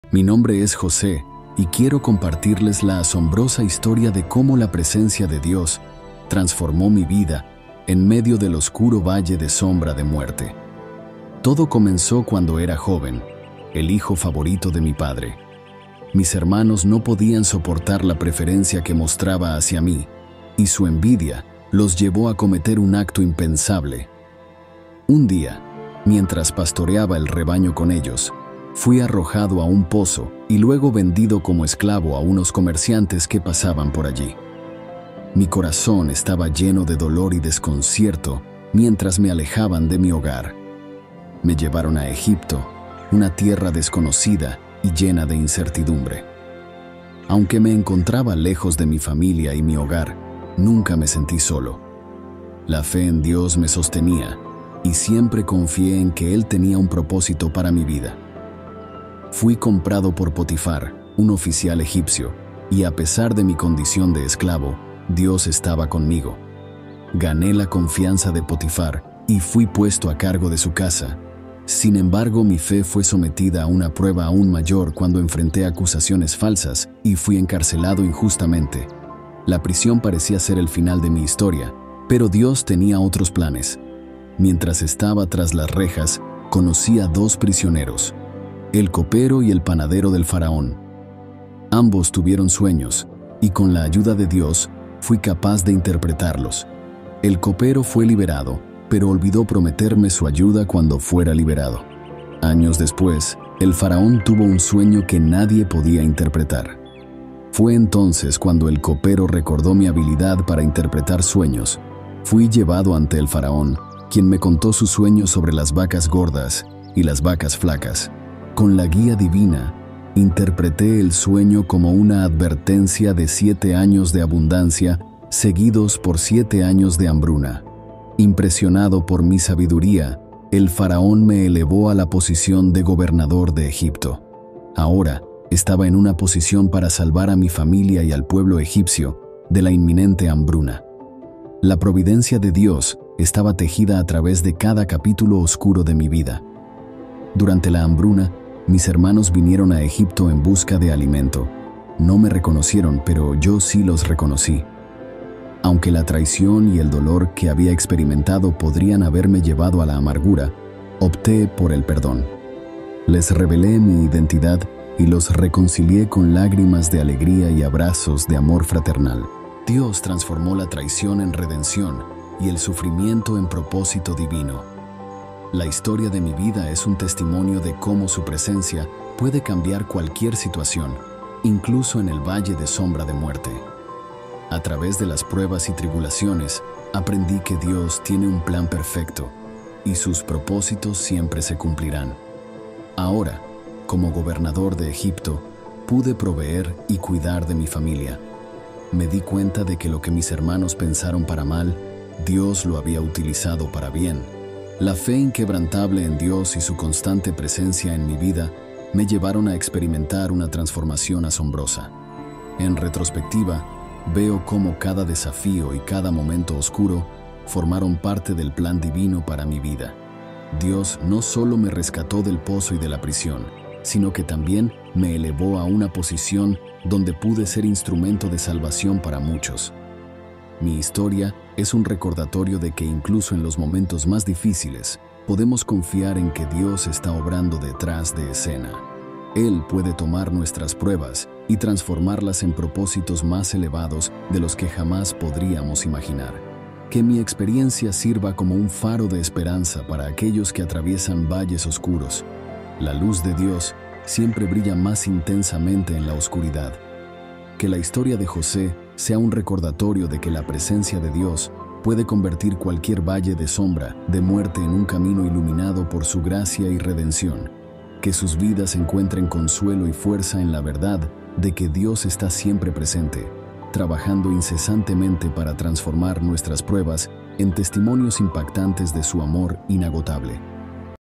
Mi Nombre es José y Esta es Mi Historia
🌊 Sumérgete: en el testimonio inspirador de JosédeEgipto, donde las profundidades del pozo se convierten en el crisol de la fortaleza humana."

🏆 TriunfoForjado: "Desde las alturas del poder, descubre cómo cada triunfo de mi travesía fue forjado en la fragua de desafíos insuperables."

🔗 CadenasTransformadas: "Explora conmigo cómo las cadenas de la esclavitud y la prisión se transformaron en puentes hacia un destino inimaginable."

💡 FarosDeEsperanza: "Acompáñame en este relato, donde los sueños se erigen como faros guía, iluminando los senderos de la resiliencia y la esperanza en los momentos más oscuros.